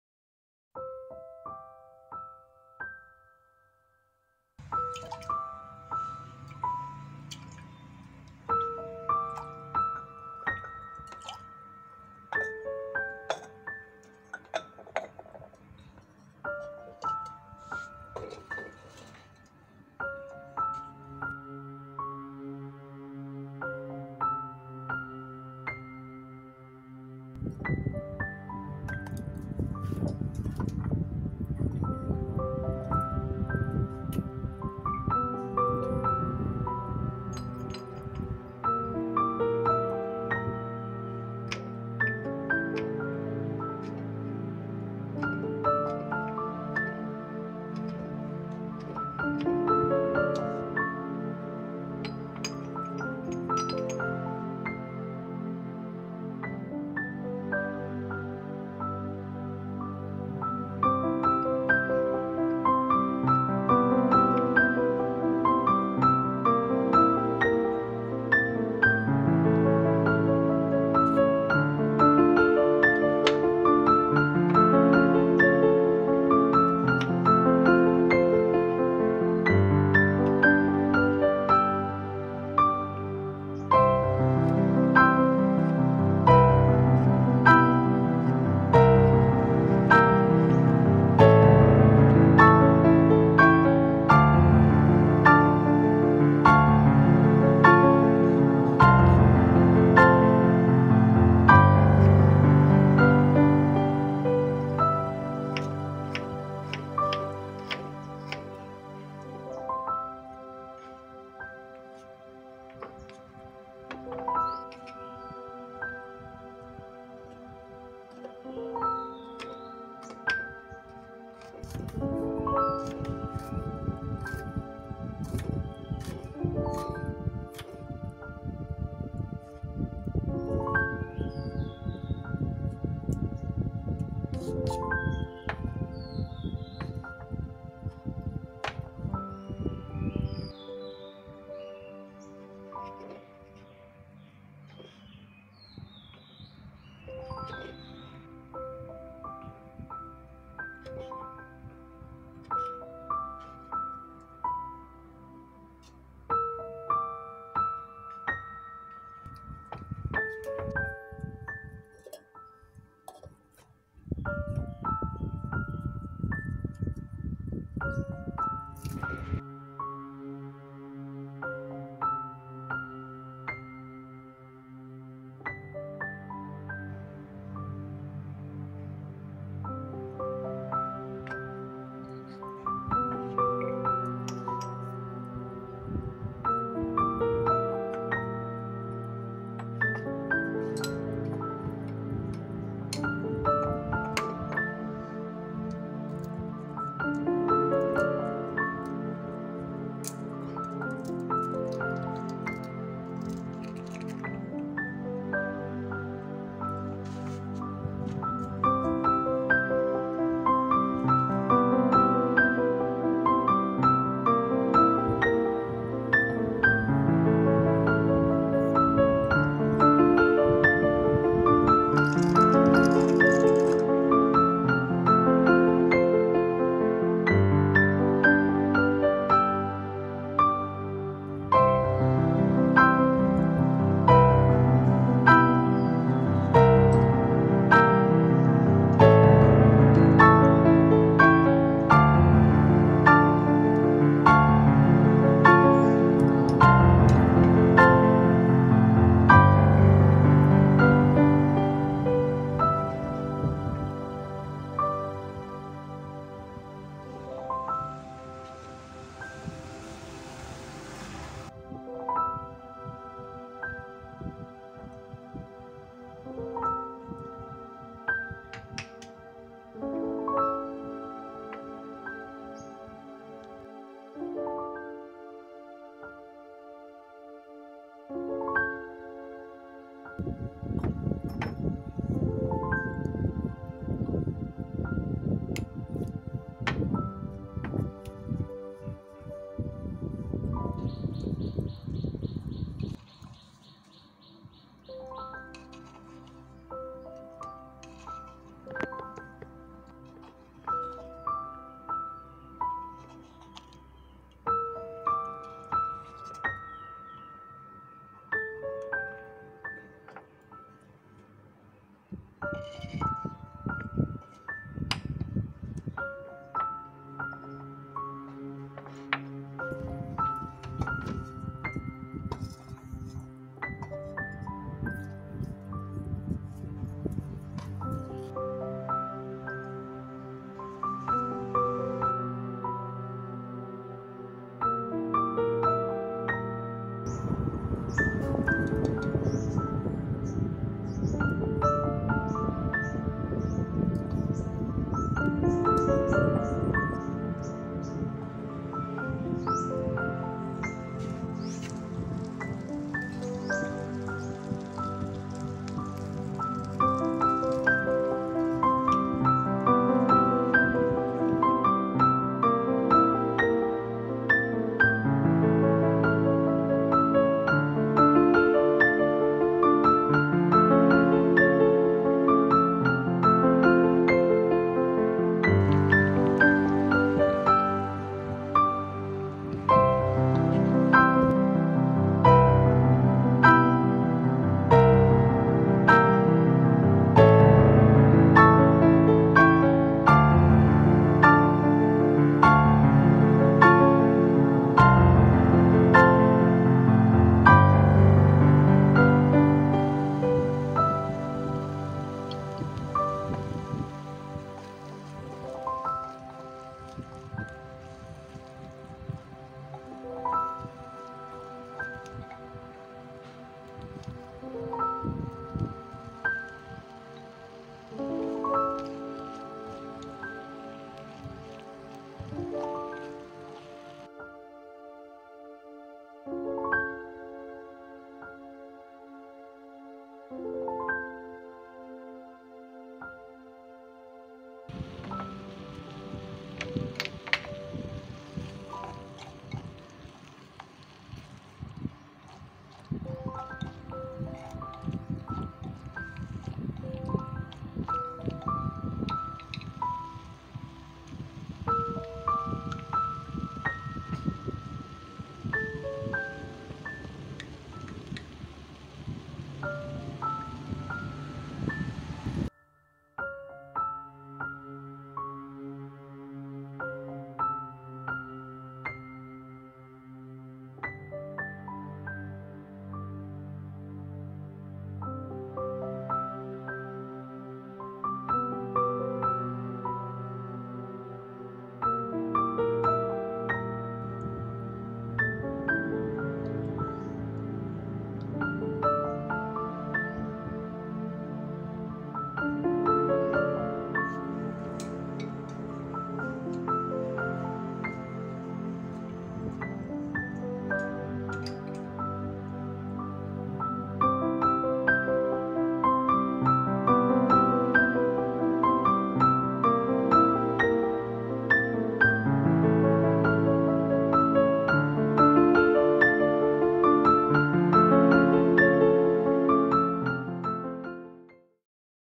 Stir Fried Pasta with Shrimp Recipe/ Mini Real Cooking with Miniature Clay Pottery
Mini Real Cooking Stir Fried Pasta with Shrimp Recipe with Miniature Clay Pottery
ผัดมักกะโรนีกุ้ง